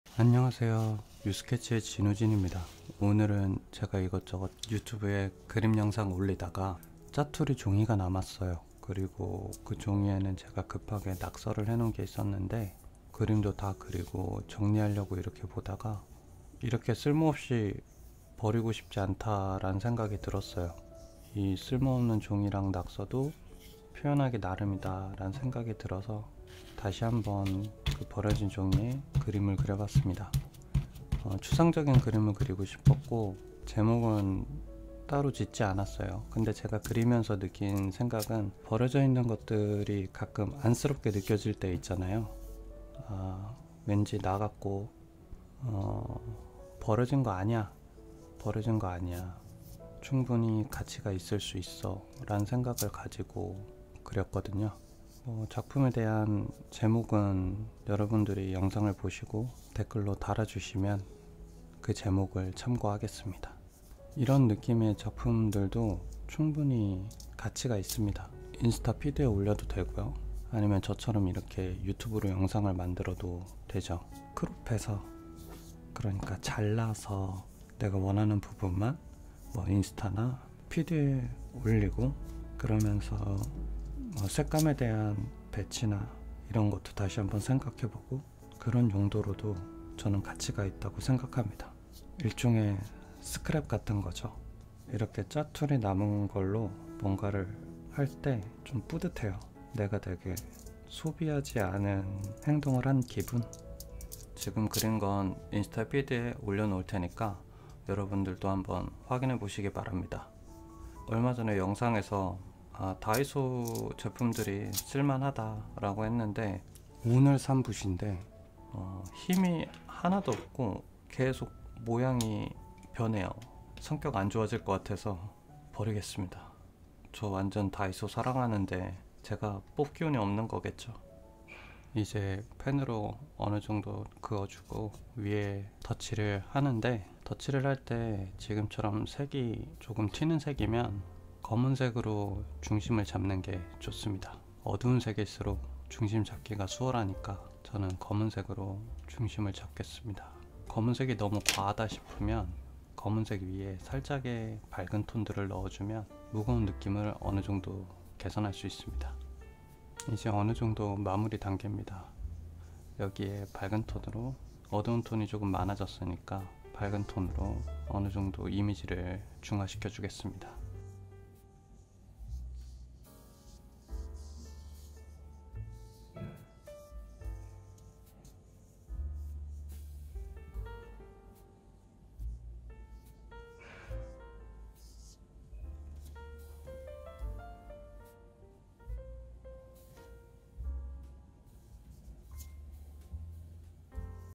다이소 아크릴 물감으로 그림그리기 (아크릴화, 추상화)
쓸모 없다고 생각하는 낙서도 쓸모가 있을 수 있어요...
내가 어떻게 생각하고 어떻게 쓰느냐가 중요한거죠.

제가 산 다이소 붓은...어떻게 쓸 수가 없네요.ㅅ...